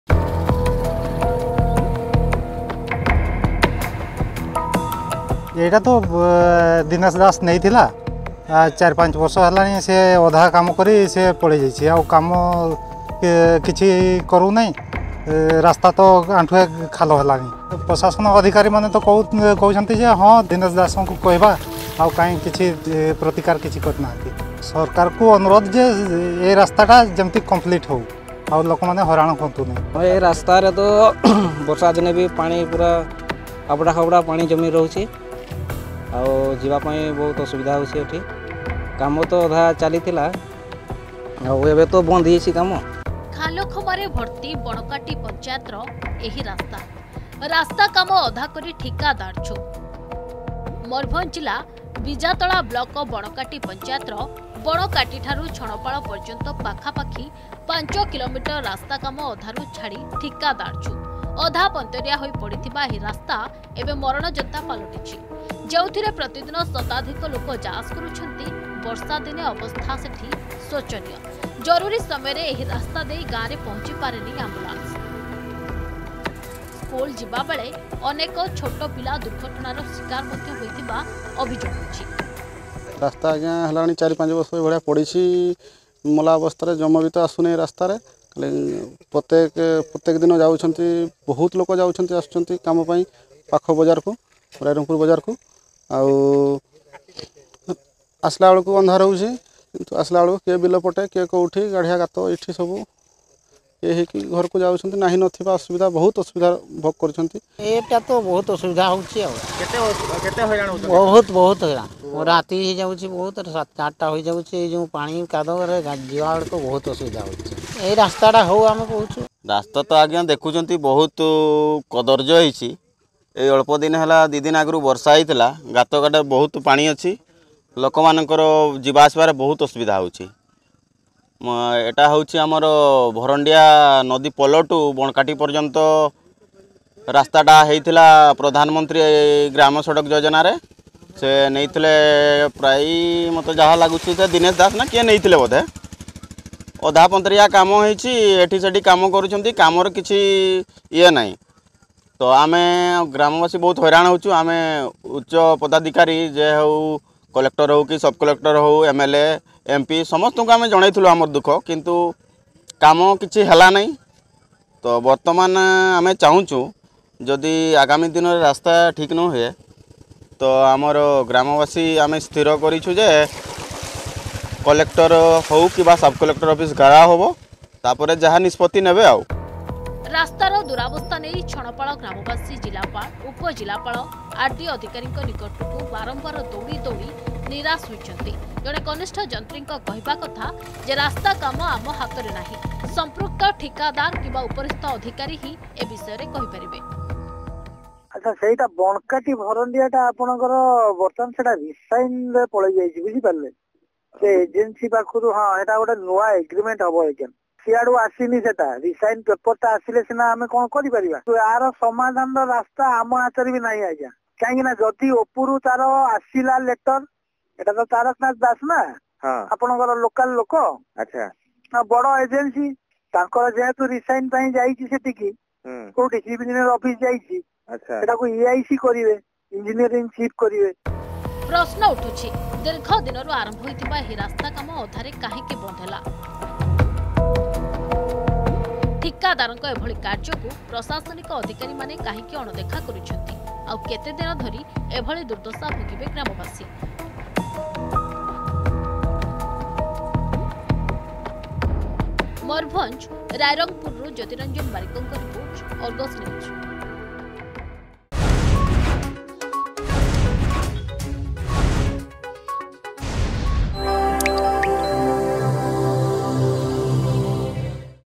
ଖାଲଖମାରେ ଭର୍ତ୍ତି ବଣକାଟି ପଞ୍ଚାୟତର ରାସ୍ତା।
ଖାଲଖମାରେ ଭର୍ତ୍ତି ବଣକାଟି ପଞ୍ଚାୟତର ରାସ୍ତା।  ରାସ୍ତା କାମ ଅଧା କରି ଠିକାଦାର ଛୁ। ମୟୁରଭଞ୍ଜ ଜିଲ୍ଲା ବିଜାତଳା ବ୍ଳକ ବଣକାଟି ପଞ୍ଚାୟତର ବଣକାଟିଠାରୁ ଛଣପାଳ ପର୍ଯ୍ୟନ୍ତ ପାଖାପାଖି 5 କିଲୋମିଟର ରାସ୍ତା କାମ ଅଧାରୁ ଛାଡି ଠିକାଦାର ଛୁ। ଅଧାପନ୍ତରିଆ ହୋଇ ପଡିଥିବା ଏହି ରାସ୍ତା ଏବେ ମରଣଯନ୍ତା ପାଲଟିଛି ।